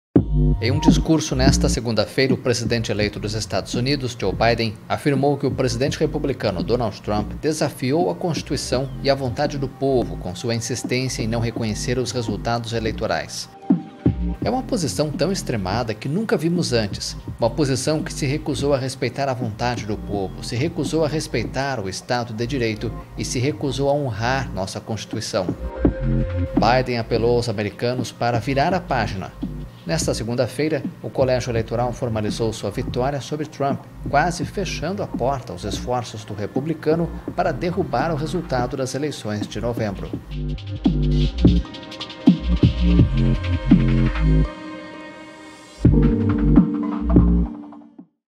Biden: Trump 'se recusou a respeitar a vontade do povo'
O presidente eleito dos Estados Unidos, Joe Biden, fez nesta segunda-feira sua maior crítica a Donald Trump desde as eleições de novembro. Confira no vídeo.